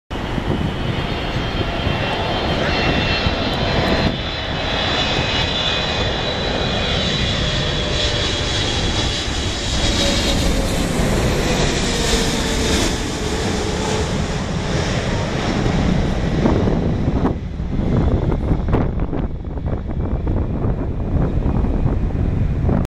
Turkish Airlines Boeing 777-300ER [TC-JJF] landing in LAX
Flight info:
Airline: Turkish Airlines
Aircraft: Boeing 777-3F2ER
Reg: TC-JJF
Livery/Name: Beylerbeyi
Airport: Los Angeles International Airport
Runway: 24R
Flight #: TK9
Route: IST-LAX
Date: 6/15/15
Time: 5:16PM
Comments: No belly sticker...